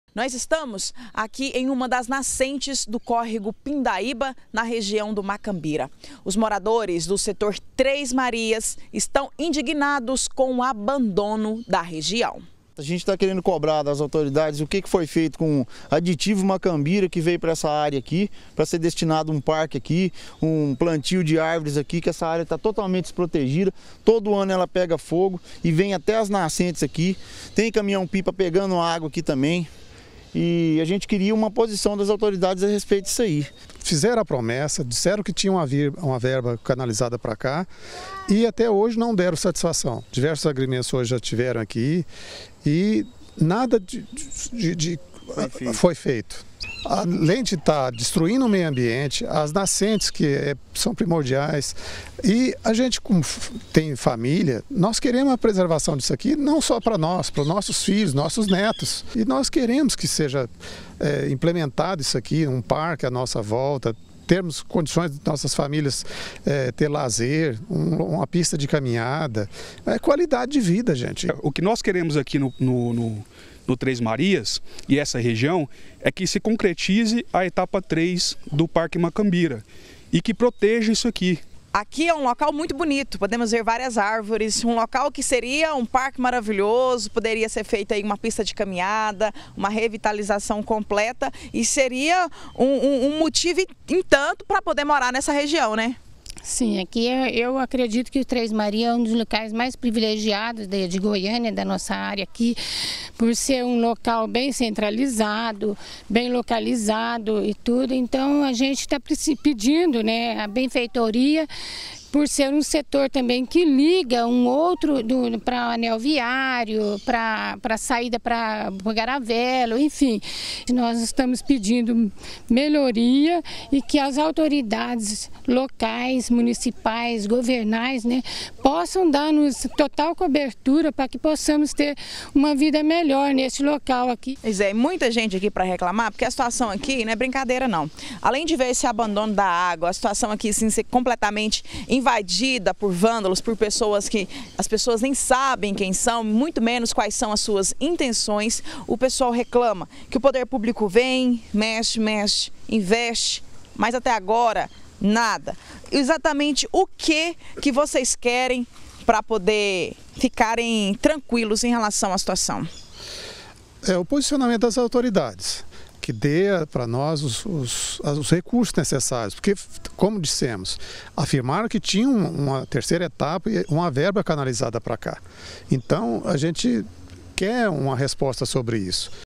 DR - Res. Três Marias: moradores denunciam abandono de área - 03-07-2018
Moradores do Residencial Três Marias estão preocupados com o abandono de uma grande área verde no bairro. Sem atenção do poder público, até as nascentes que existem no local estão comprometidas.